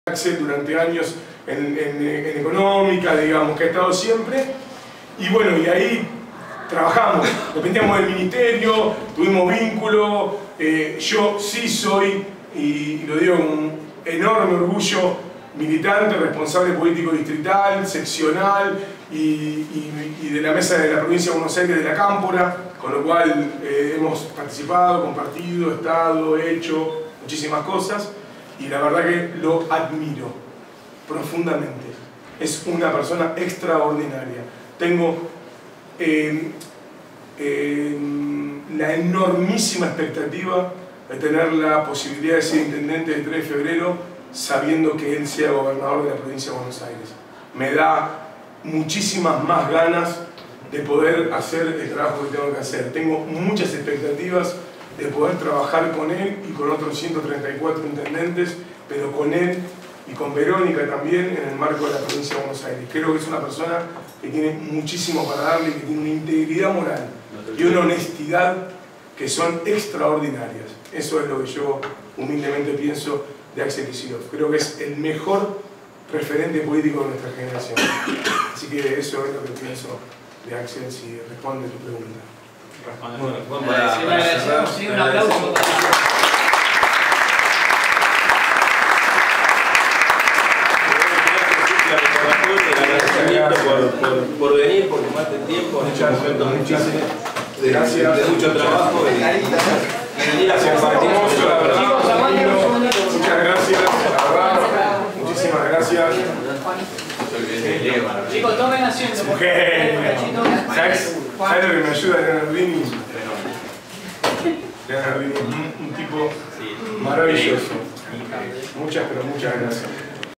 Juan Agustín Debandi en el Ciclo de Conferencias ISPeD 2019. Parte III.
El actual diputado provincial de Unidad Ciudadana visitó las instalaciones del Instituto Superior ISPeD -ubicadas en la localidad de Caseros, Partido de Tres de Febrero- para conversar en el marco del Ciclo de Conferencias ISPeD 2019.
El vecino de Pablo Podestá, se transformó en la cara del Frente de Todos en el municipio de Tres de Febrero luego del resultado en las PASO. A pesar de forcejear con una competencia de cinco contrincantes, el referente de La Cámpora triunfó con comodidad en la interna y será la cara del sello de Alberto Fernández y Cristina Fernández de Kirchner.
Es Licenciado en Administración Pública con doble titulación UNTREF en convenio con la Université de Poitiers - Francia. Pertenece a "La Cámpora" y comenzó en política de la mano del histórico Intendente Hugo Curto
Los moderadores fueron los periodistas, docentes e integrantes del ISPeD Diego Mario Pero y Martín “Bocha” Flores.
Bienvenidos a esta, la tercera y última parte donde no se deja ningún tema sin conversar.
Juan Debandi para todos ustedes.